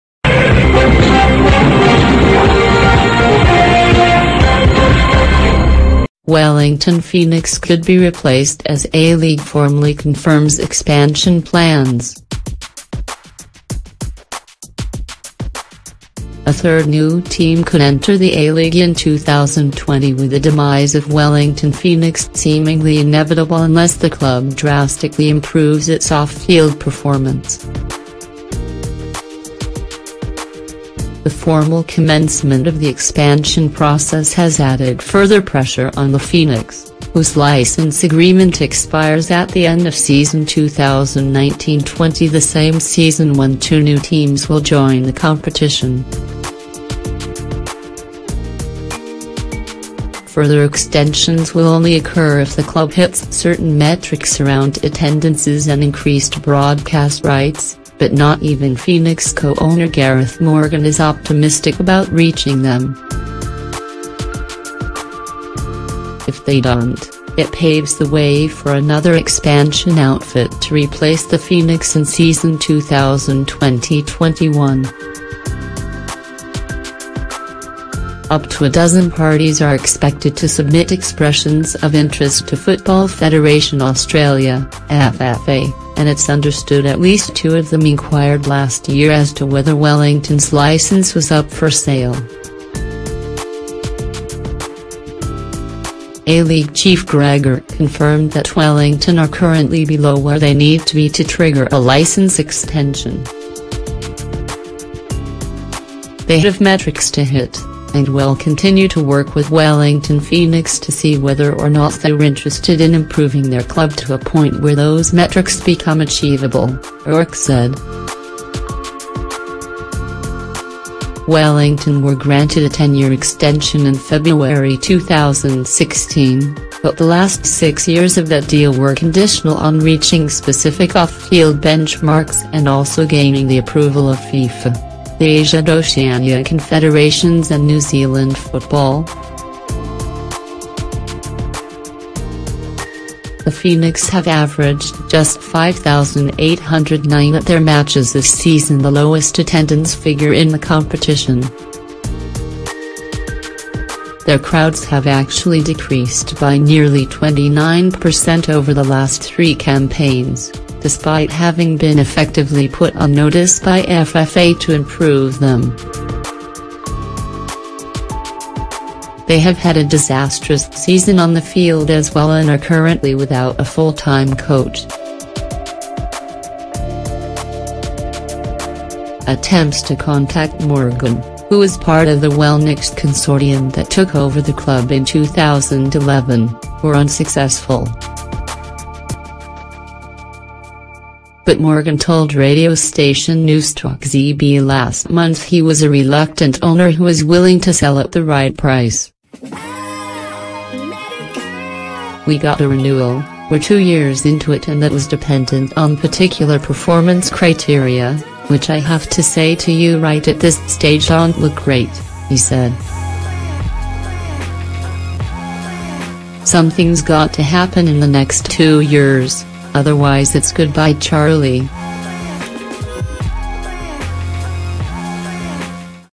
Wellington Phoenix could be replaced as A-League formally confirms expansion plans[News NZ 24h]
Wellington Phoenix could be replaced as A-League formally confirms expansion plans